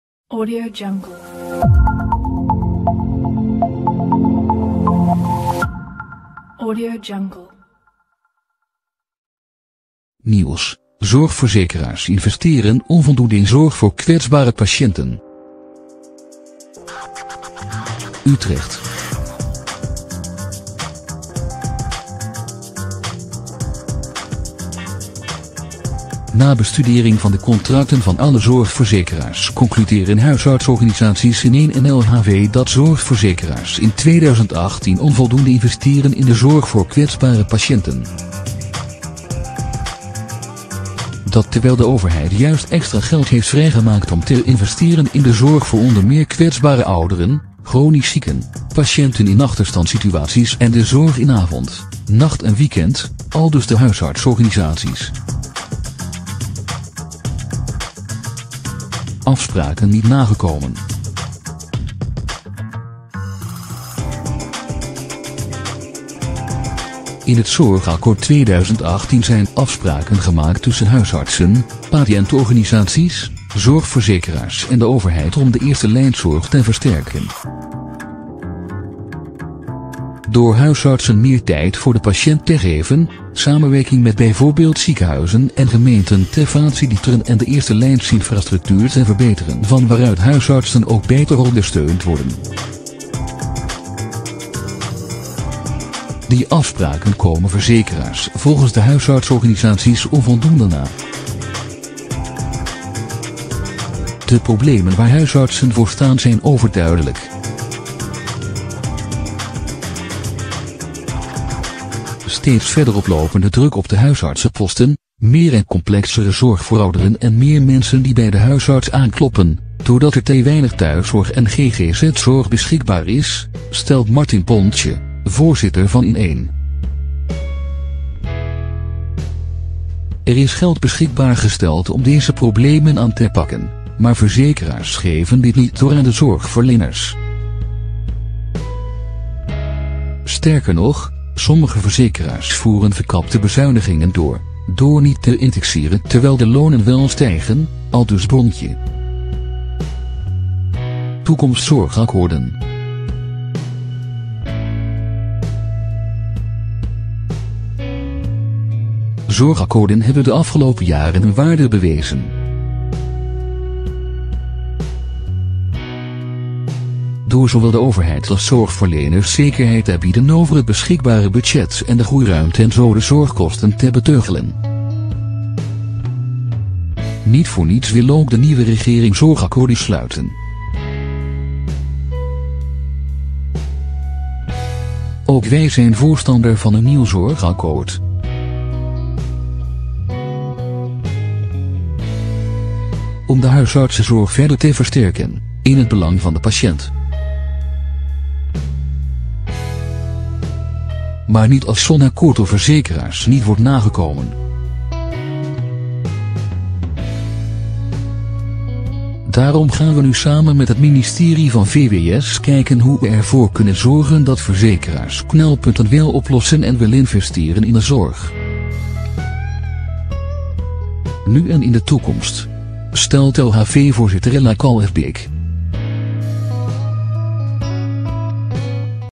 Nieuws: 'Zorgverzekeraars investeren onvoldoende in zorg voor kwetsbare patiënten'
Nieuws: 'Zorgverzekeraars investeren onvoldoende in zorg voor kwetsbare patiënten' | UtrechtNa bestudering van de contracten van alle zorgverzekeraars concluderen huisartsenorganisaties InEen en LHV dat zorgverzekeraars in 2018 onvoldoende investeren in de zorg voor kwetsbare patiënten.

'Dat terwijl de overheid juist extra geld heeft vrijgemaakt om te investeren in de zorg voor onder meer kwetsbare ouderen, chronisch zieken, patiënten in achterstandssituaties en de zorg in avond, nacht en we